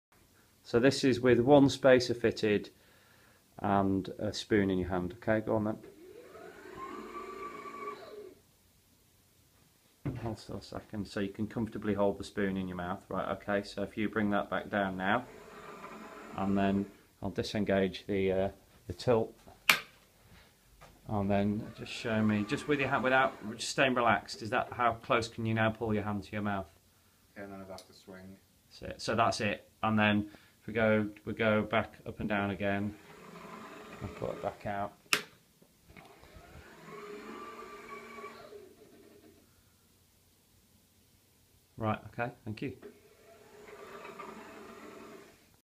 Neater Arm Support: Tilt and lock Upgrade | Neater Solutions
Showing the enhanced ability of engaging the tilting and locking bracket on the Neater arm support with and without its use for comparison.